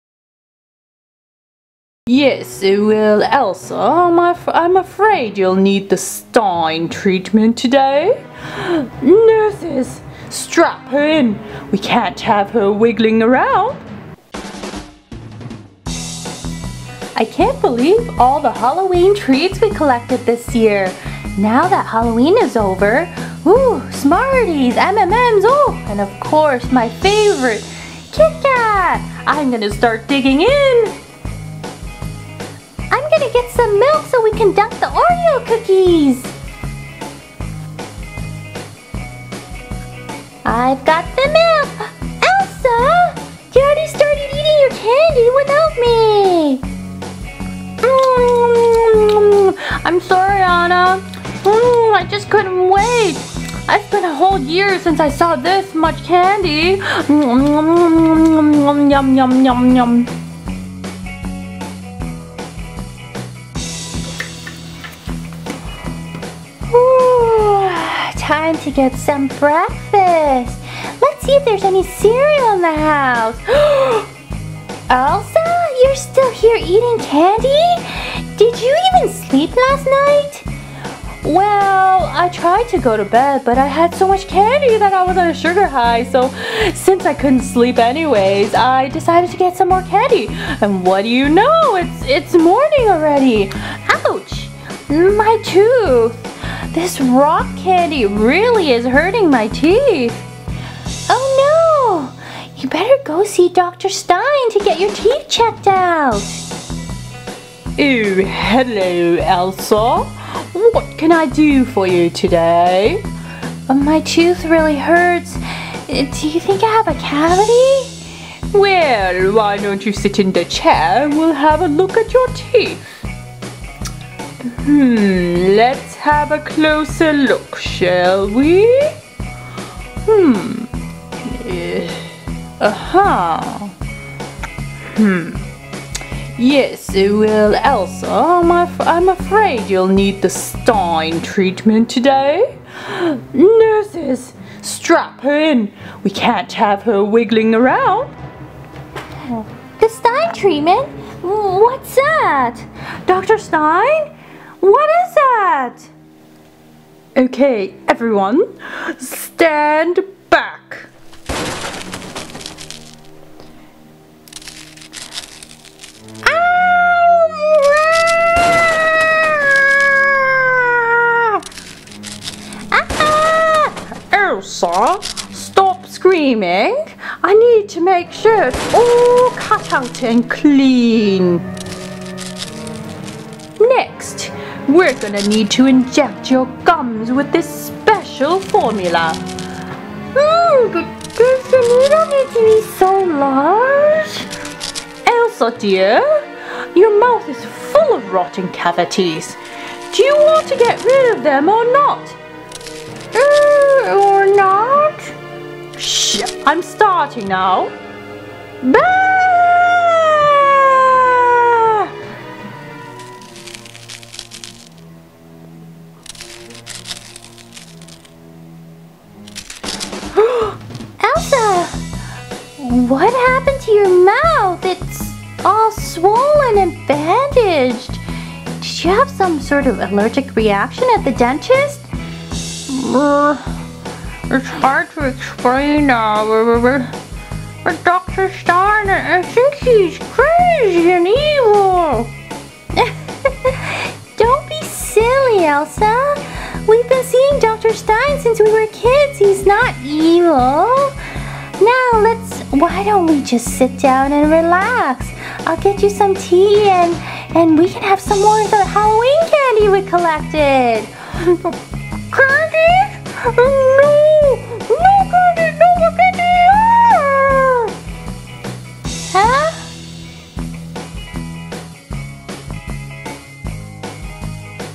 Frozen ELSA Goes to the Dentist after Halloween! Funny superheroes in real life kid's story
Elsa has too much Halloween candy and needs to go to the dentist!

All images from Pixabay.